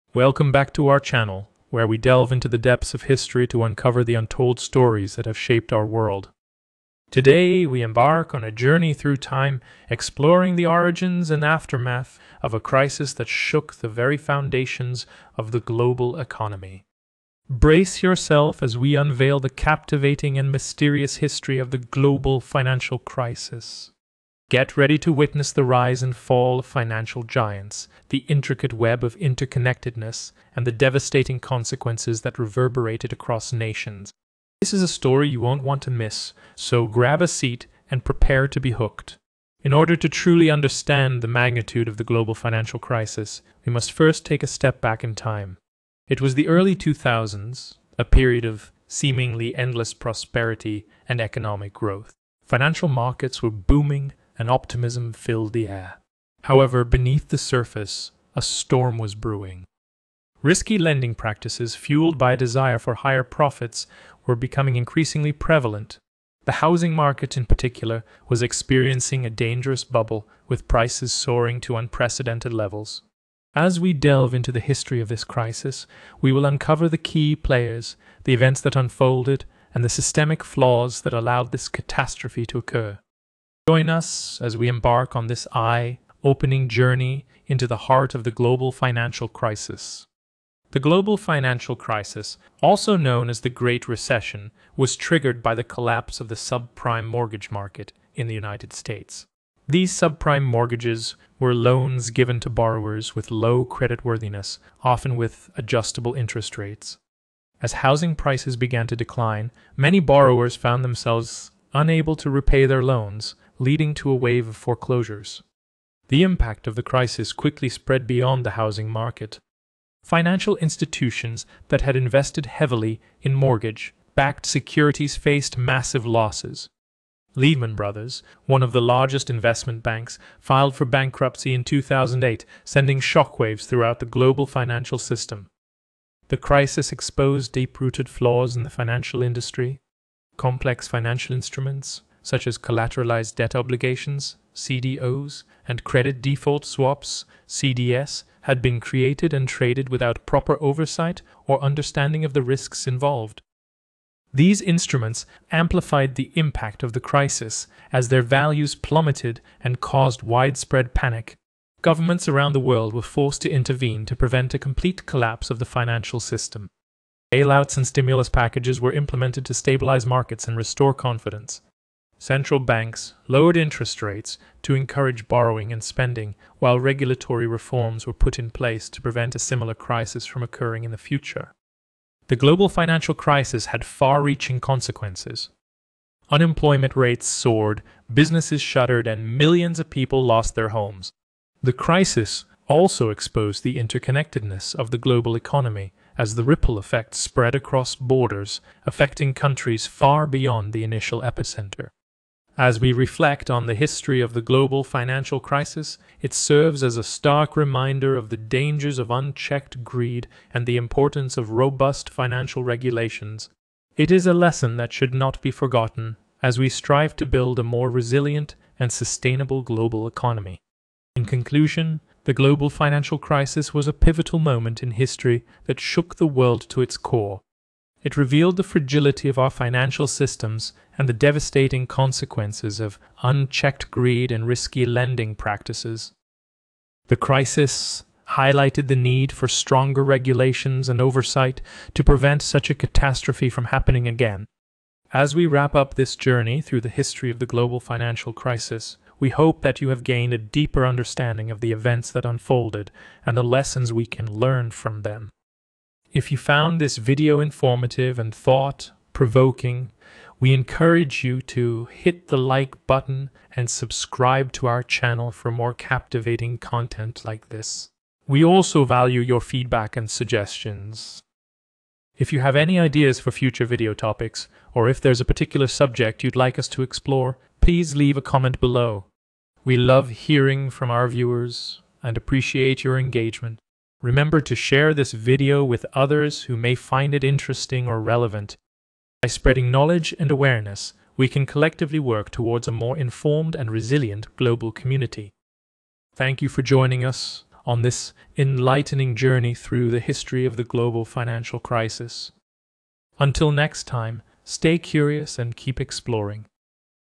The History of the Global Financial Crisis
Contents:
00:45 - Chapter 1: Origins of the Global Financial Crisis
01:05 - Chapter 2: The Housing Bubble and Risky Lending Practices
01:30 - Chapter 3: The Collapse of the Subprime Mortgage Market
02:06 - Chapter 4: The Impact on Financial Institutions
02:18 - Chapter 5: The Role of Complex Financial Instruments
02:60 - Chapter 6: Consequences and Lessons Learned

Discover the untold story behind the worldwide economic turmoil that shook nations to their core. Uncover the key events, shocking revelations, and lessons learned from The History of the Global Financial Crisis that will leave you enlightened and prepared for the future. Don't miss out on this eye-opening journey through the past!